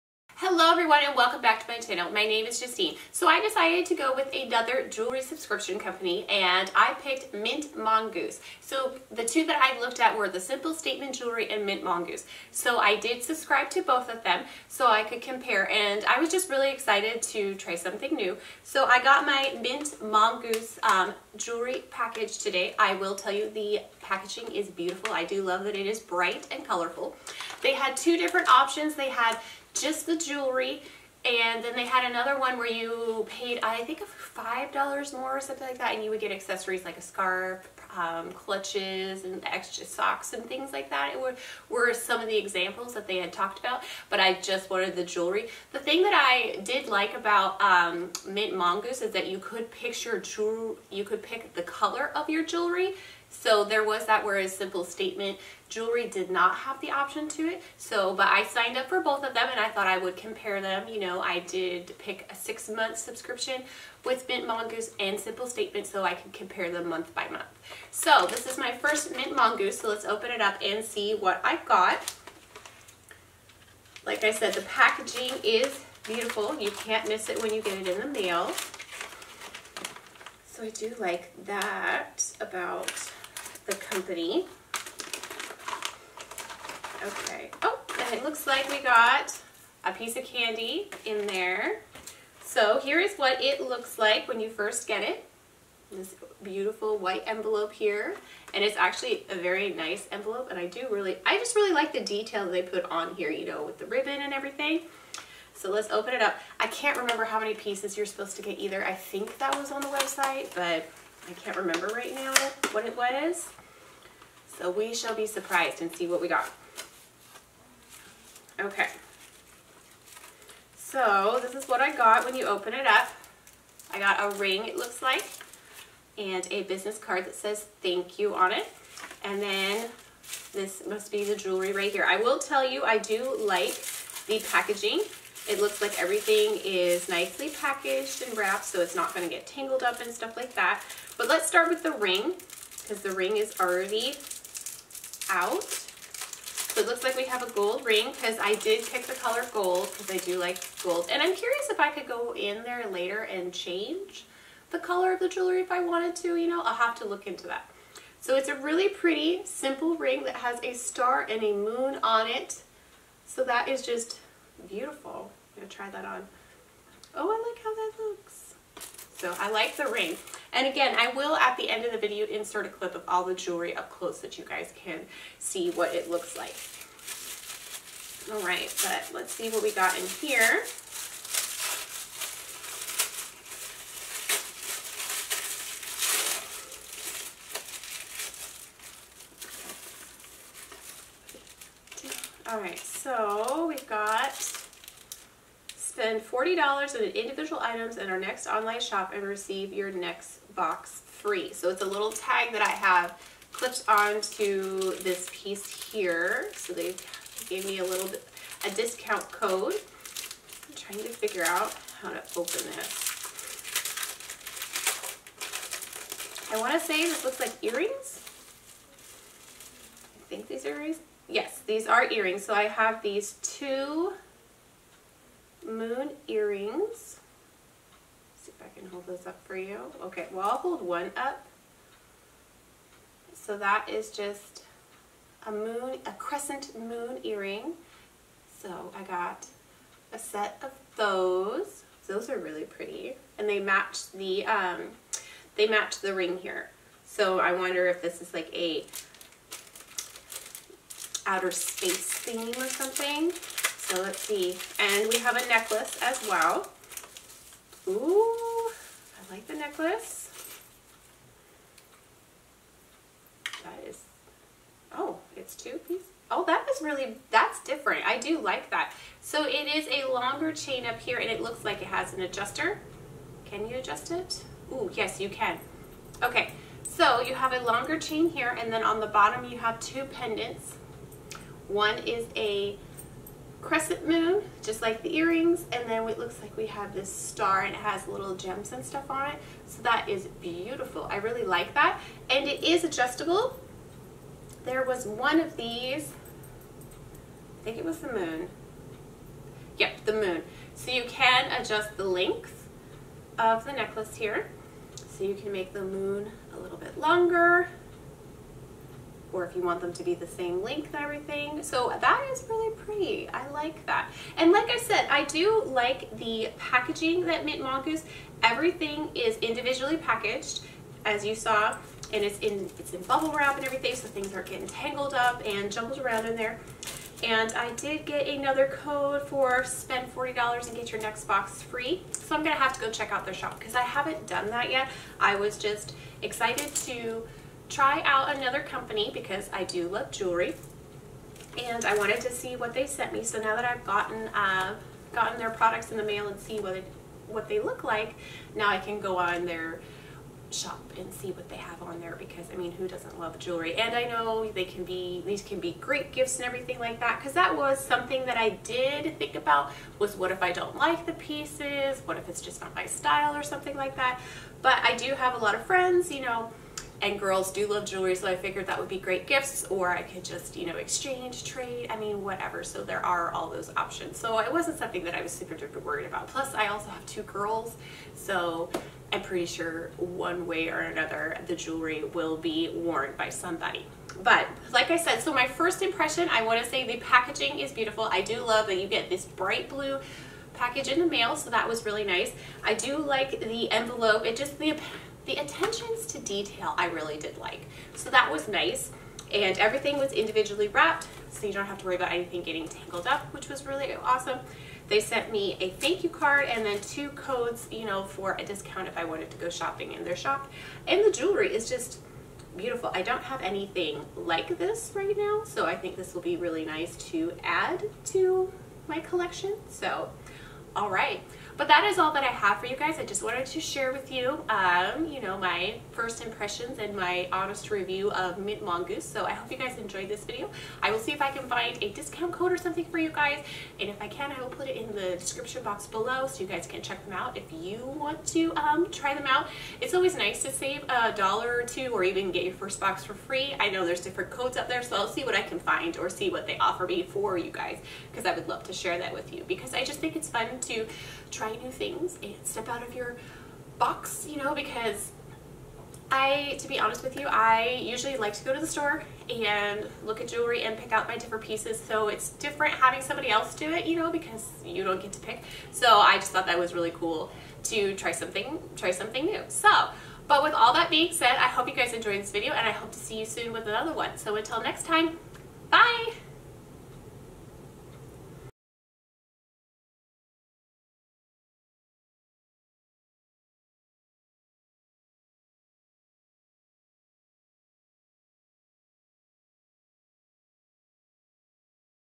My first Mongoose Jewelry Subscription box
I decided to try another jewelry subscription company and I chose Mint Mongoose! Watch and see what I got!!